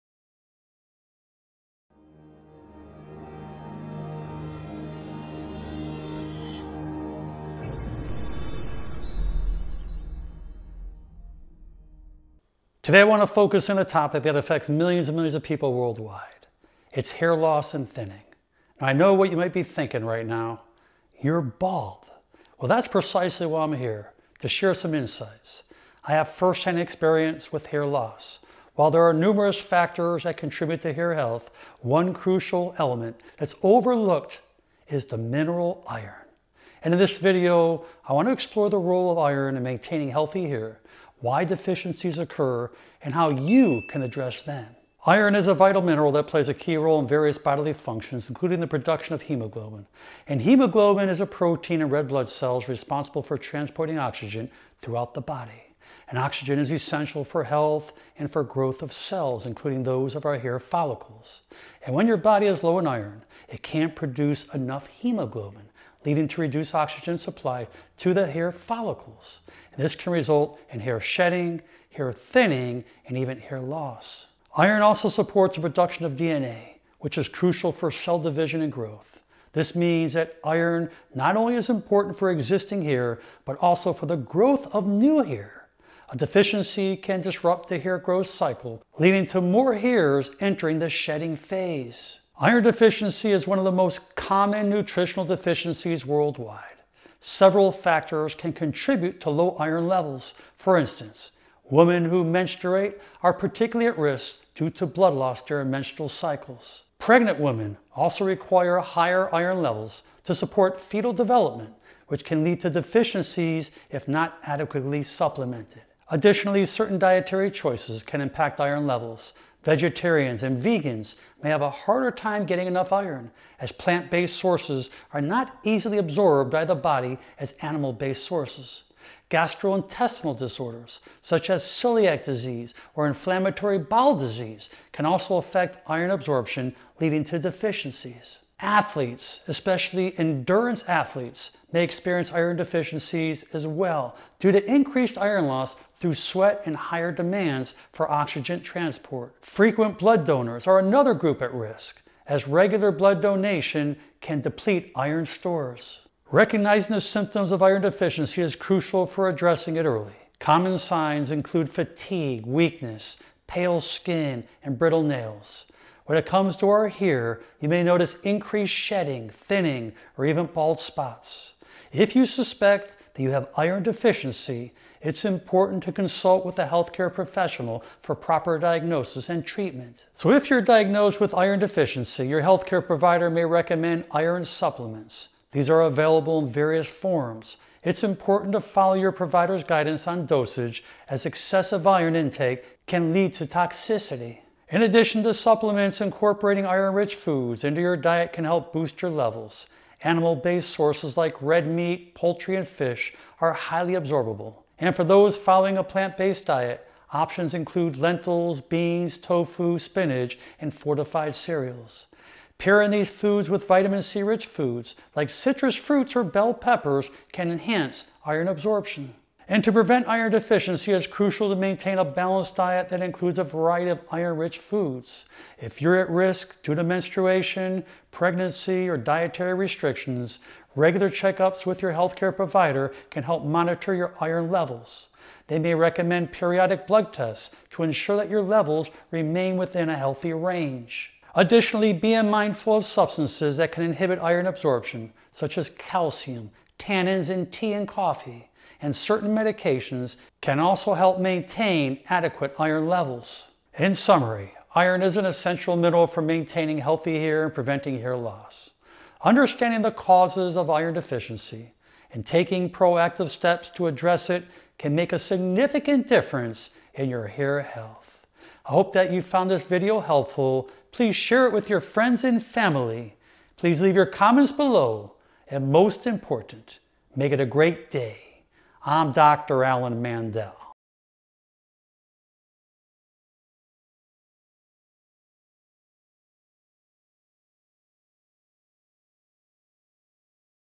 The 1 Mineral You’re Missing That Causes Hair Loss! Dr. Mandell
Hey everyone! I just wrapped up a video on a topic that affects many of us: iron deficiency and its link to hair loss. Iron is one of the most common mineral deficiencies worldwide, and it can play a sneaky role in hair thinning. Now, before you point out the obvious—yes, I'm bald! But that just gives me a unique perspective on the whole hair loss journey. 😄

Iron is a vital mineral that plays several crucial roles in our body. It's essential for producing hemoglobin, the protein in red blood cells that carries oxygen from our lungs to the rest of our body. This oxygen transport is critical not only for overall health but also for the growth and repair of cells, including those in our hair follicles. When iron levels are low, our hair follicles may not receive the oxygen they need, leading to hair shedding and thinning.

Baldness can result from a variety of factors, including genetics, hormonal changes, and lifestyle, but understanding the impact of iron is crucial for those looking to maintain healthy hair. Iron also supports the production of DNA, which is necessary for cell division and growth, making it important for both existing hair and new hair growth.

If you're curious about how iron affects hair growth or want to learn more about tackling hair loss, check out the video. It's packed with insights into the physiology of iron and practical tips for maintaining optimal levels. Let's dive into this hair-raising topic together!